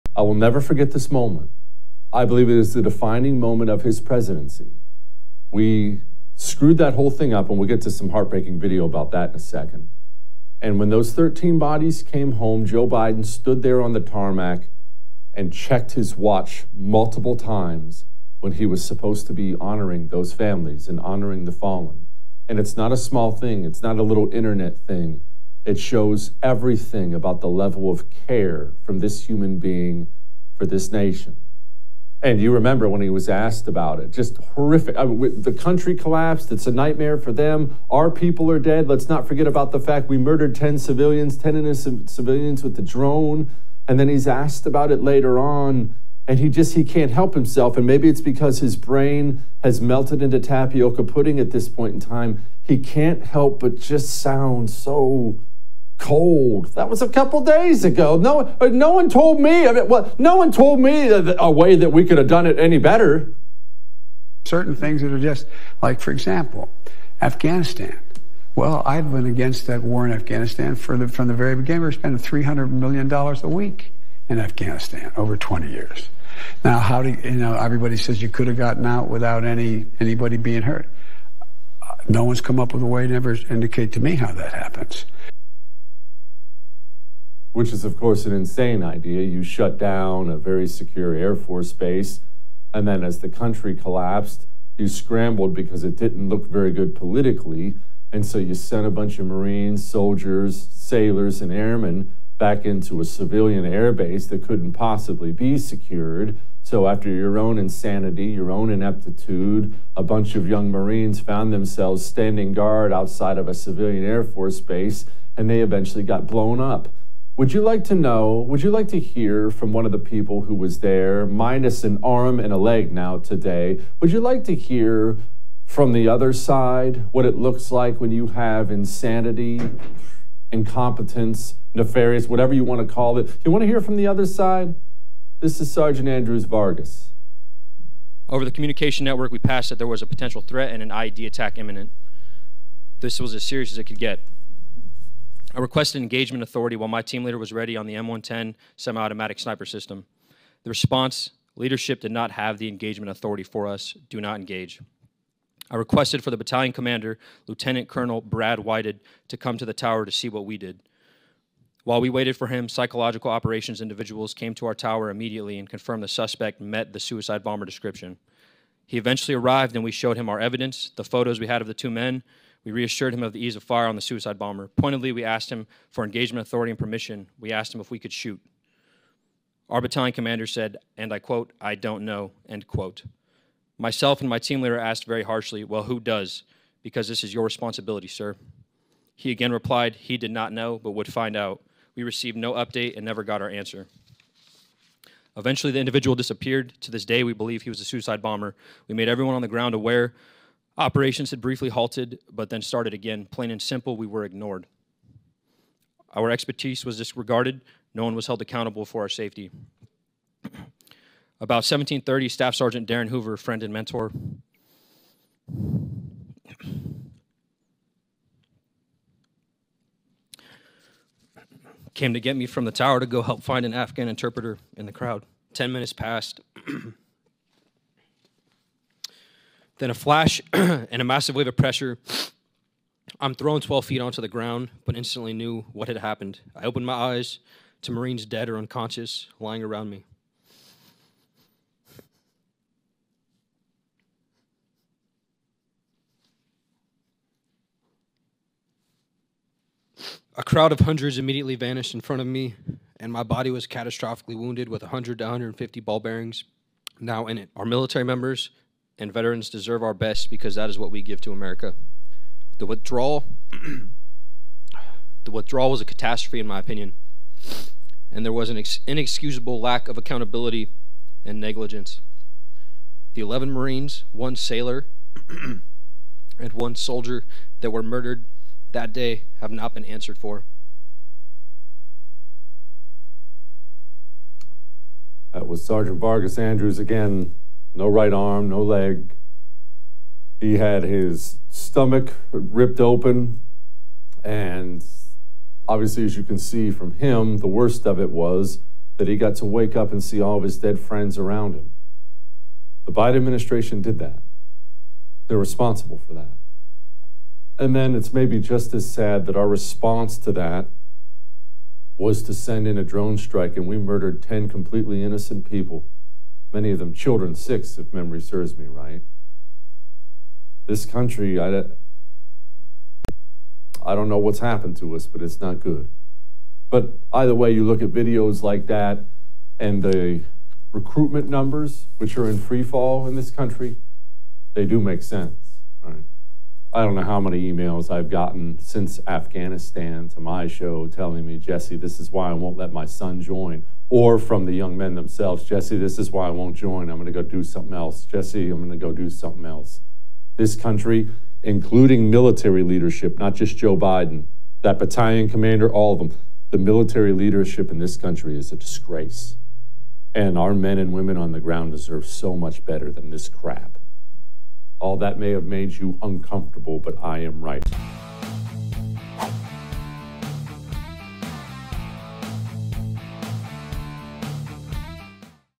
President Biden's Defining Moment
There is one moment that will forever define Joe Biden as president. That moment came after the Afghanistan withdrawal. Jesse Kelly shares that moment as well as a heartbreaking testimony from a marine who barely survived the pullout.